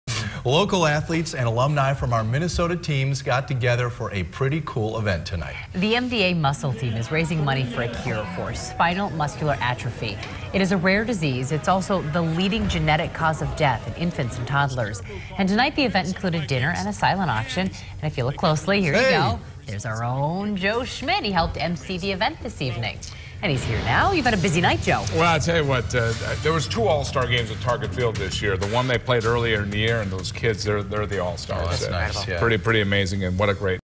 Top Minn. Athletes Help Fight Muscle Disease
Local athletes and alumni from our Minnesota teams got together for an event to help fight muscle disease Tuesday night. The Muscular Dystrophy Associated Muscle Team is raising money for a cure for Spinal Muscular Atrophy. It's a rare, disease but also the leading genetic cause of death in infants and toddlers.